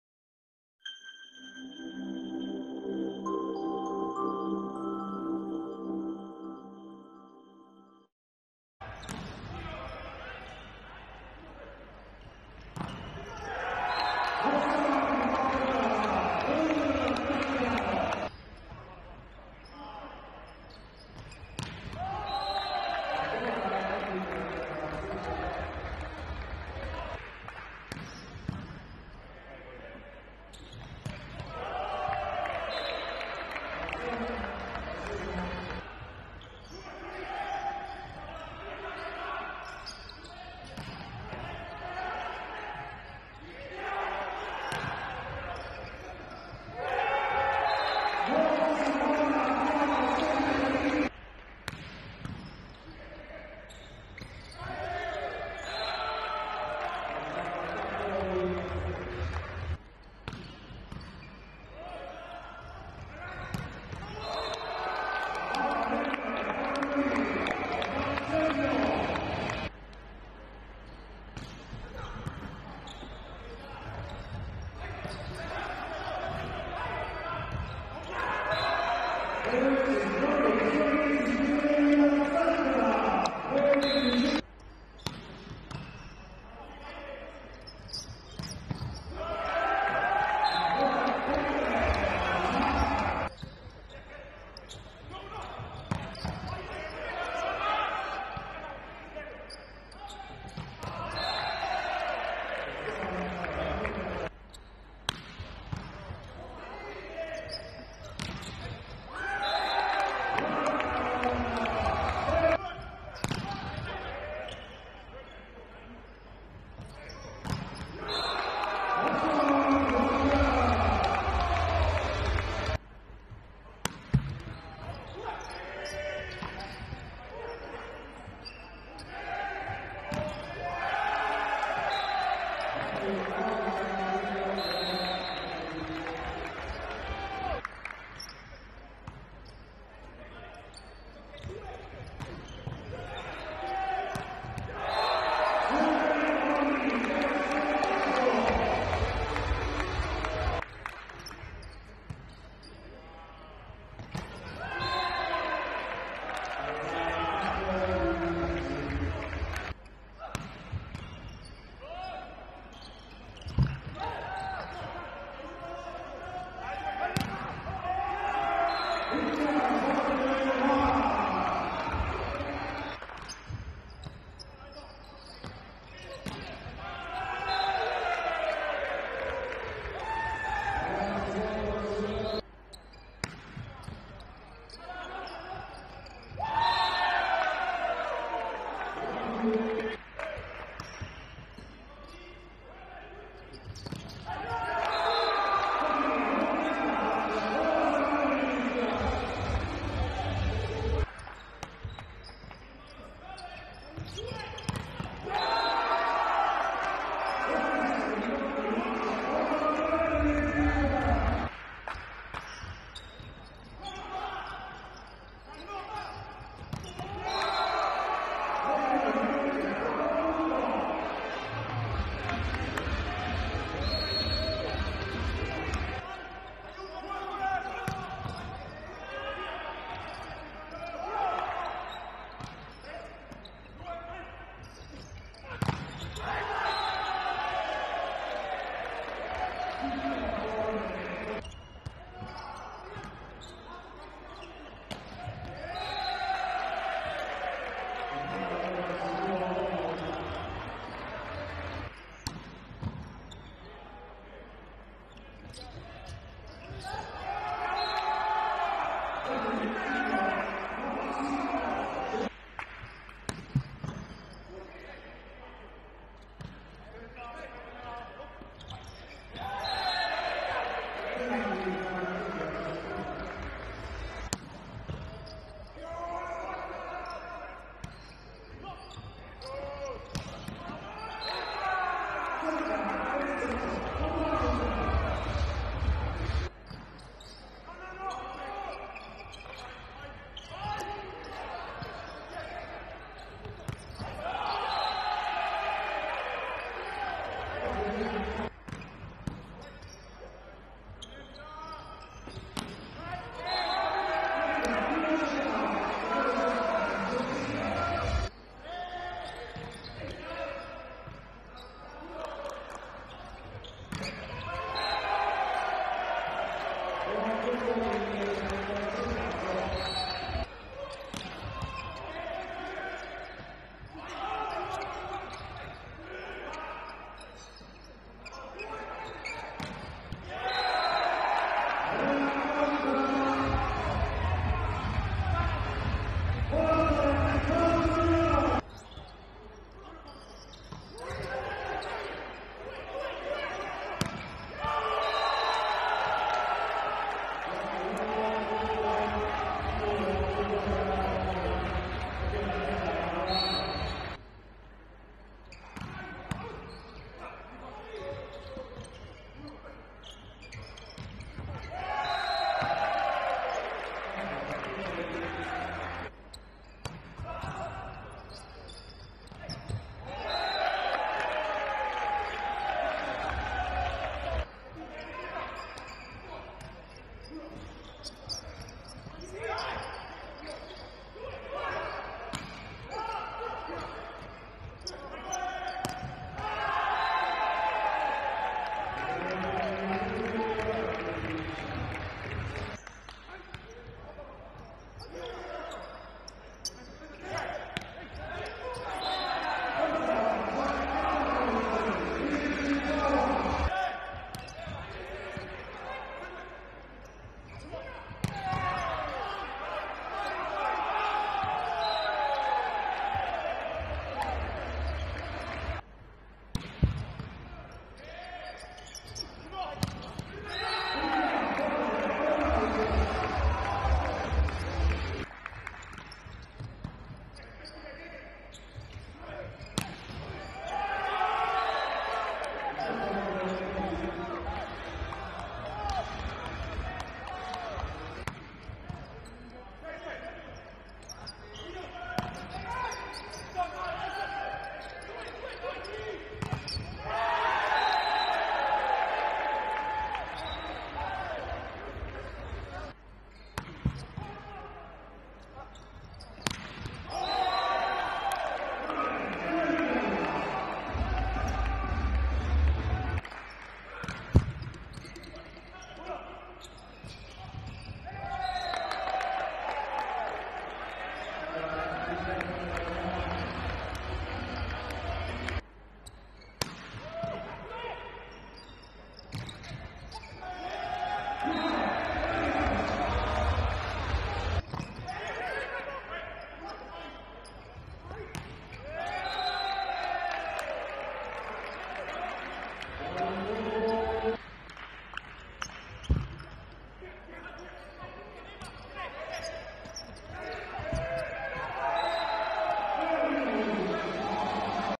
Playoff 5° posto, highlights Milano - Modena
📹 | HIGHLIGHTS
Rivivi gli highlights della sfida tra Allianz Powervolley Milano e Leo Shoes Modena.